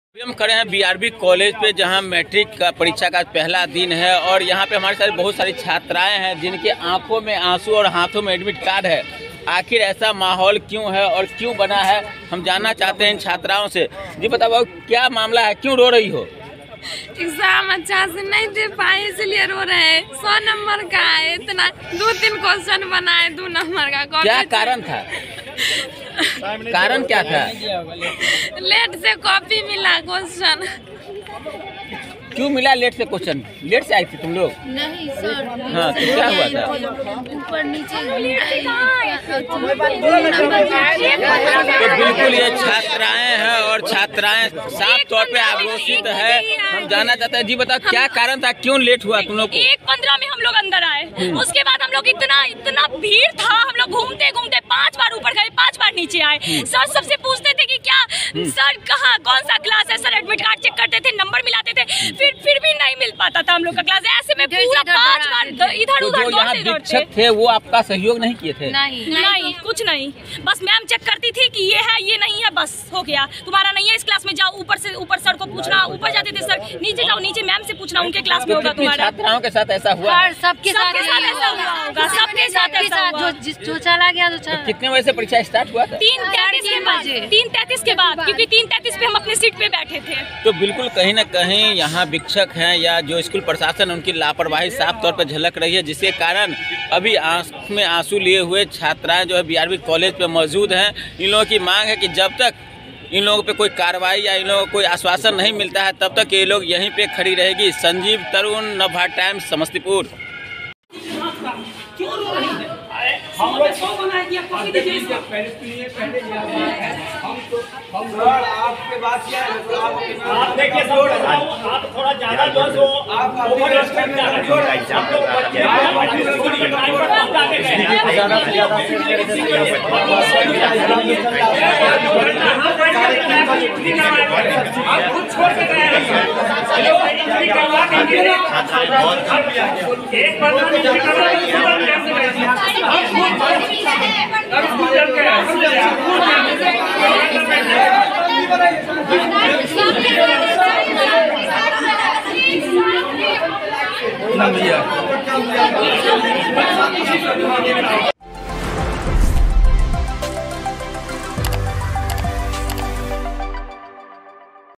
'पेपर देर से मिला...' और रो पड़ीं बिहार बोर्ड 10वीं की छात्राएं, BRB कॉलेज में हंगामा, Watch Video
बिहार बोर्ड की दसवीं की परीक्षा गुरुवार से शुरू हो गई। इसी बीच समस्तीपुर में दूसरी पाली की परीक्षा के दौरान बीआरवी कॉलेज हंगामा देखने को मिला। यहां परीक्षा देने पहुंची छात्राओं का आरोप है कि समय से प्रश्न पत्र नहीं मिलने के कारण उन लोगों को काफी दिक्कत हुई। उन्हें दोपहर 3:30 बजे पेपर दिया गया और 5:00 बजे छीन लिया गया। जिसके कारण वह सही से प्रश्न पत्र का जवाब नहीं दे सकीं। छात्राओं ने कॉलेज प्रशासन पर आरोप लगाए कि पेपर एक ही समय पर दिया जाता है लेकिन हमारे साथ ऐसा नहीं किया गया।